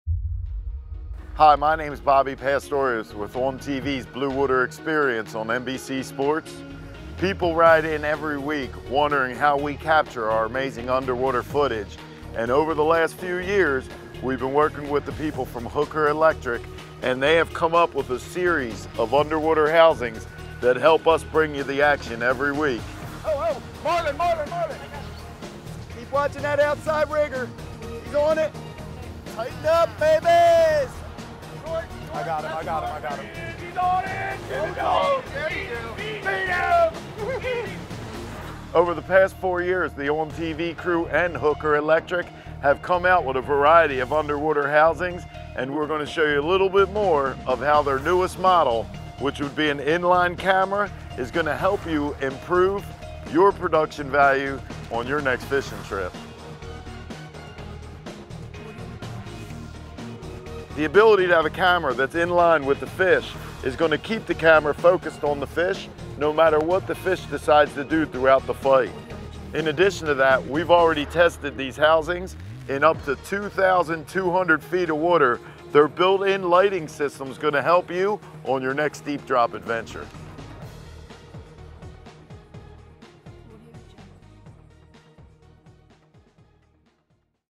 Hooker Electric Camera Housing
A brief video show casing the underwater housings of Hooker Electric Inc.